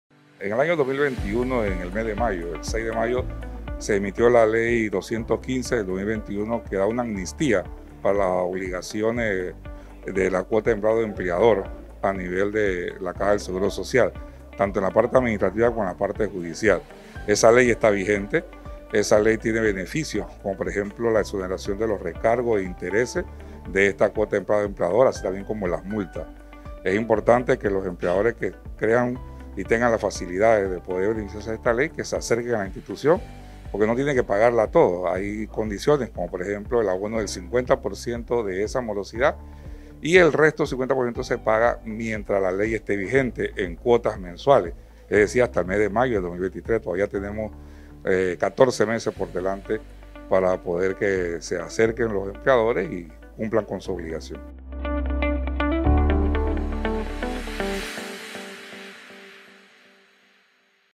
CSS insta a empresarios morosos, se acerquen y se beneficien con Ley de Amnistía
Un llamado al sector empresarial que mantiene una morosidad con la Caja de Seguro Social (CSS), para que se acerquen a la entidad y a través de la Ley de Amnistía de mayo de 2021, hagan arreglos de pago y se puedan poner al día con la institución.

El Lcdo. Roberto A. Cueto Cisneros, coordinador administrativo judicial de la entidad de seguridad social, precisó que esta ley tiene beneficios al empleador, ya que le permite hacer un pago inicial del 50% de la deuda, exonerar los pagos de recargos e interés y multas.

Solamente el año pasado, en el área judicial, aclaró el Lcdo. Cueto, se manejaron 1947 expedientes a través de esta ley, de ellos, 317 empleadores cancelaron sus deudas y recibieron 1,100 abonos de morosidad pendiente.

Con ello se alcanzó durante el 2021 una recaudación en la jurisdicción coactiva de 4.519,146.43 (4 millones, 519 mil 146 balboas con 43 centavos, sostuvo el coordinador administrativo judicial.

“Esta es una oportunidad que tienen los contribuyentes que adeudan a la institución para que se acerquen, cumplan con su obligación y se eviten situaciones de orden penal”, destacó el Lcdo. Cueto Cisneros.

Dicha amnistía de dos años aplica a las personas naturales, jurídicas, trabajadores independientes, contribuyentes, cotizantes voluntarios y trabajadores que reciben salarios y honorarios que se encuentran moroso con la CSS hasta el 31 de marzo de 2021.

A través de esta medida, quienes mantienen una morosidad pueden hacerse el abono del 50% de la deuda y en lo que resta de la videncia de la ley que se extiende hasta el 7 de mayo de 2023.